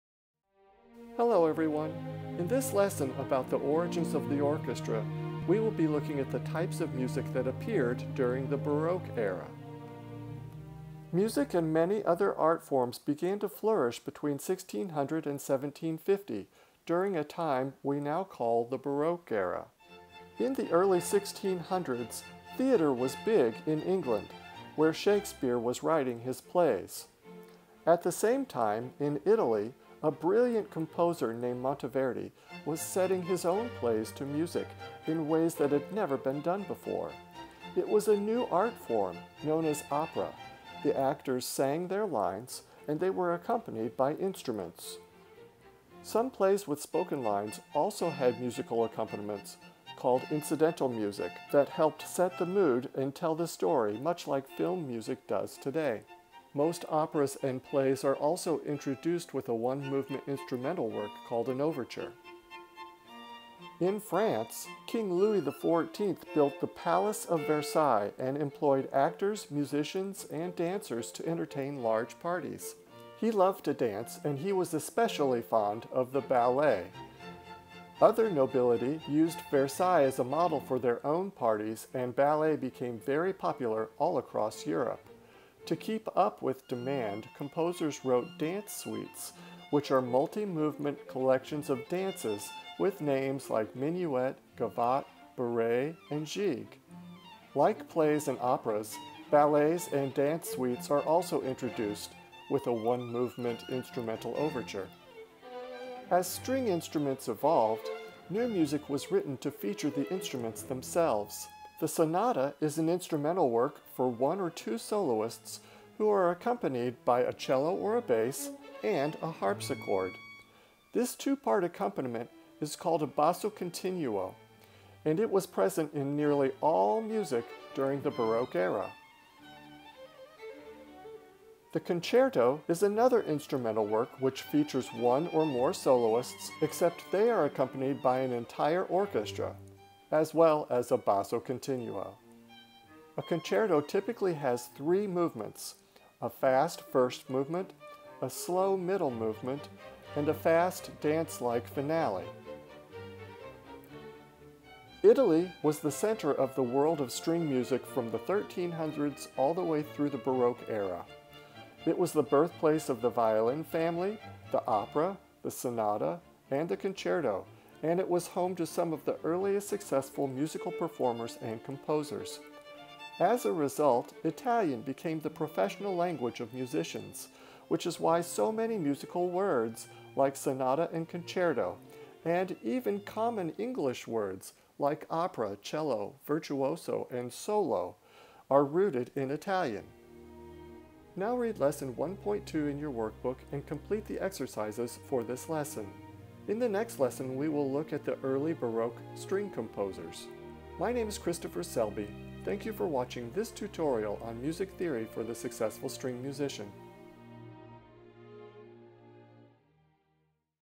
1.2 The Baroque Era (1600-1750)
Free educational video about the types of orchestra music that began to appear during the Baroque Era.